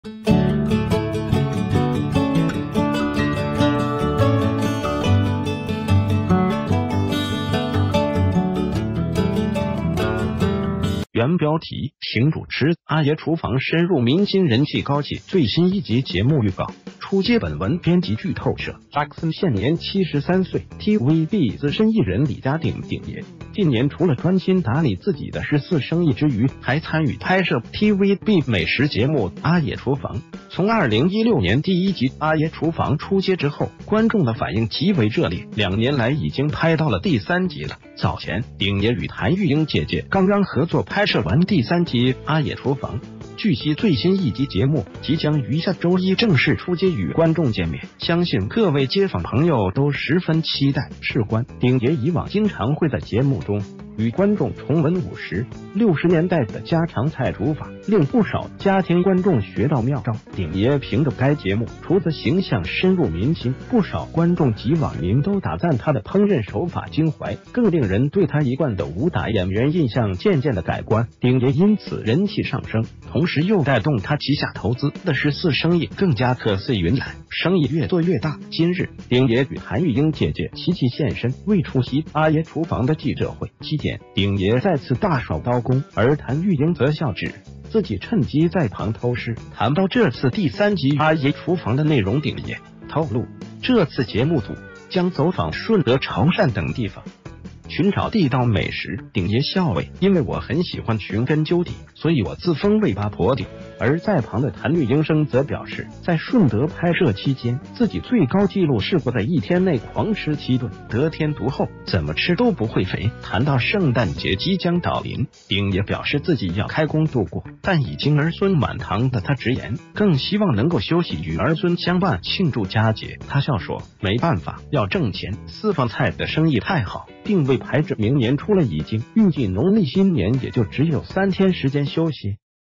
憑主持《阿爺廚房》深入民心人氣高企 最新一輯節目預告出街
谢谢！

      原標題：憑主持《阿爺廚房》深入民心人氣高企 最新一輯節目預告出街
            本文編輯劇透社：Jackson
?現年73歲TVB資深藝人李家鼎（鼎爺），近年除了專心打理自己的食肆生意之余，還參與拍攝TVB美食節目《阿爺廚房》，從2016年第一輯《阿爺廚房》出街之后，觀眾的反應極為熱烈，兩年來已經拍到了第三輯了。

早前，鼎爺與譚玉瑛姐姐剛剛合作拍攝完第三輯《阿爺廚房》，據悉最新一輯節目即將于下周一正式出街與觀眾見面，相信各位街坊朋友都十分期待。事關，鼎爺以往經常會在節目中與觀眾重溫50、60年代的家常菜煮法，令不少家庭觀眾學到妙招。

鼎爺憑著該節目，廚子形象深入民心，不少觀眾及網民都打贊他的烹飪手法精淮，更令人對他一貫的武打演員印象漸漸地改觀，鼎爺因此人氣上升，同時又帶動他旗下投資的食肆生意更加客似云來，生意越做越大。

今日，鼎爺與譚玉瑛姐姐齊齊現身，為出席《阿爺廚房》的記者會。期間，鼎爺再次大耍刀功，而譚玉瑛則笑指自己趁機在旁偷師。

談到這次第三輯《阿爺廚房》的內容，鼎爺透露這次節目組將走訪順德潮汕等地方，尋找地道美食。鼎爺笑謂：「因為我很喜歡尋根究底，所以我自封為「八婆鼎」。」
而在旁的譚玉瑛聲則表示：「在順德拍攝期間，自己最高紀錄試過在一天內狂吃七頓，得天獨厚，怎么吃都不會肥。」

談到圣誕節即將到臨，鼎爺表示自己要開工度過，但已經兒孫滿堂的他直言，更希望能夠休息與兒孫相伴慶祝佳節。他笑說：「沒辦法，要掙錢，私房菜的生意太好，訂位排至明年初了已經，預計農歷新年也就只有3天時間休息。」